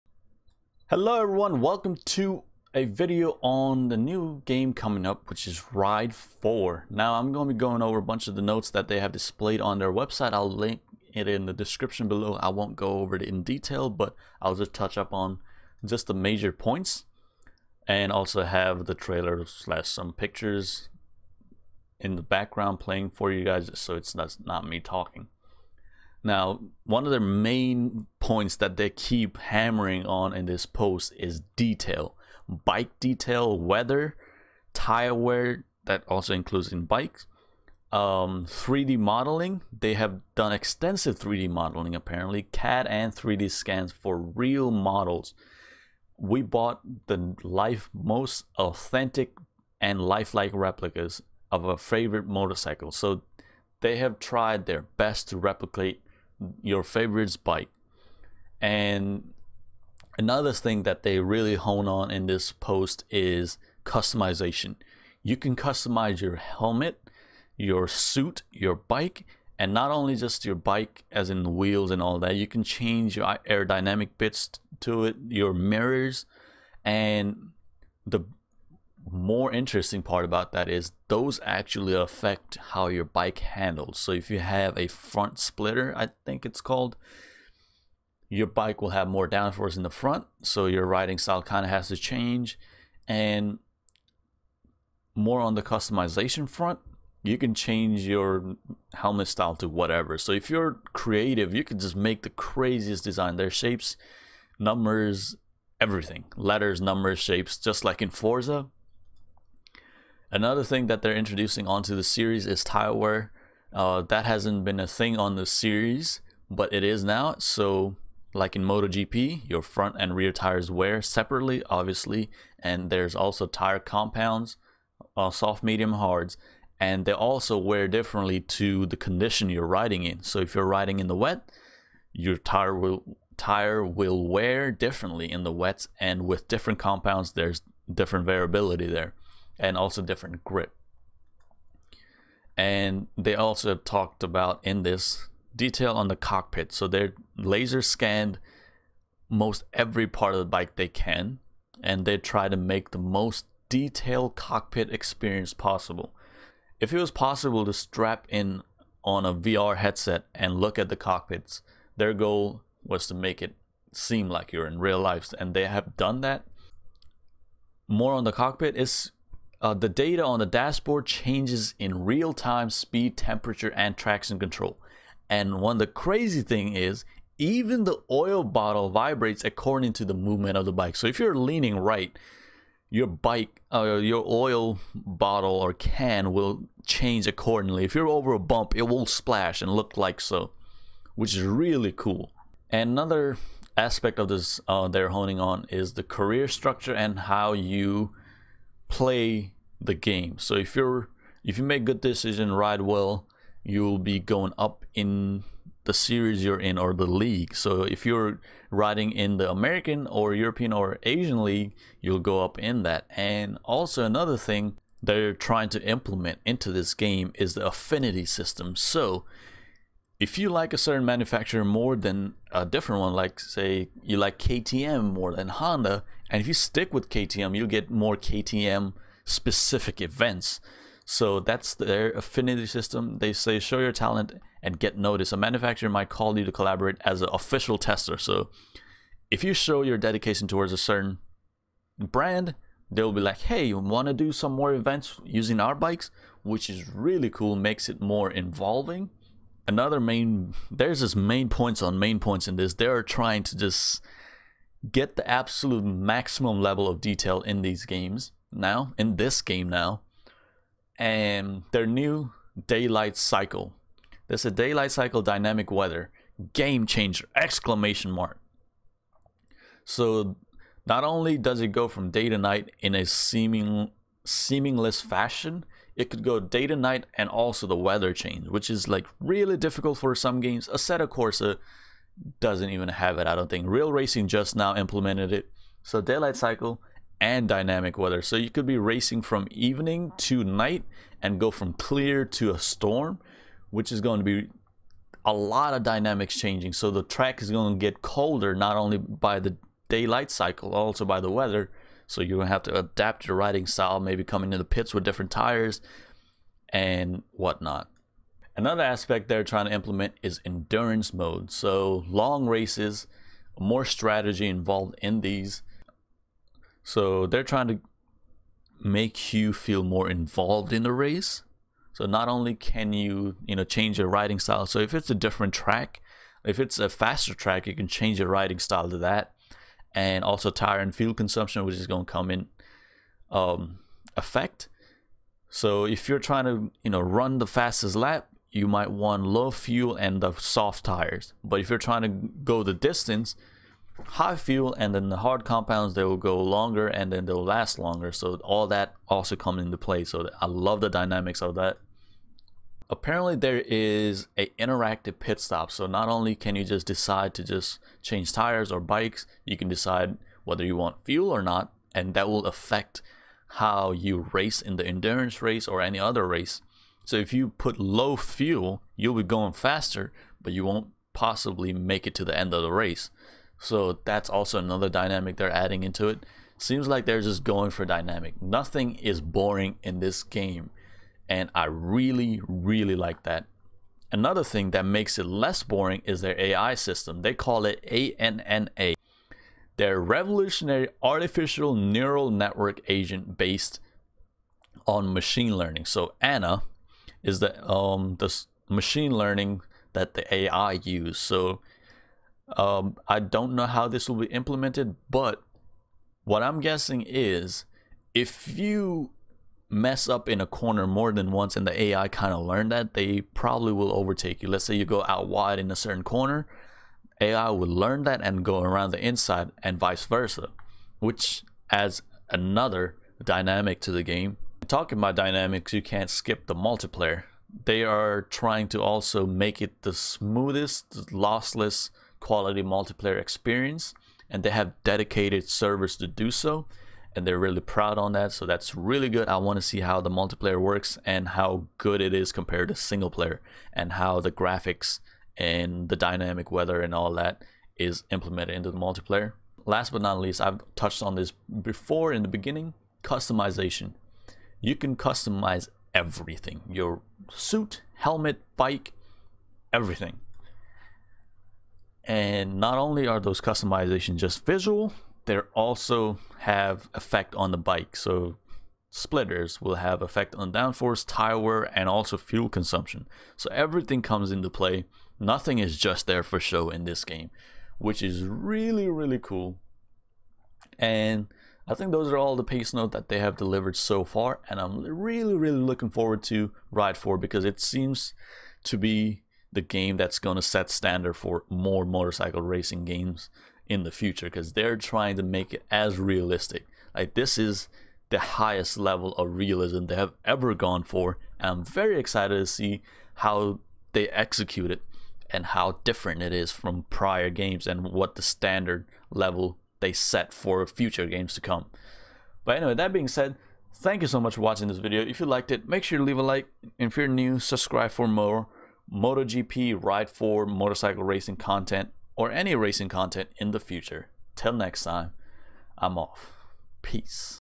THIS GAME WILL CHANGE EVERYTHING!! RIDE 4!!
From what i have read so for this game will set the standered for future motorcycle racing games! the attention to detail they claim is unlike anything i have heard or seen in any other game before!!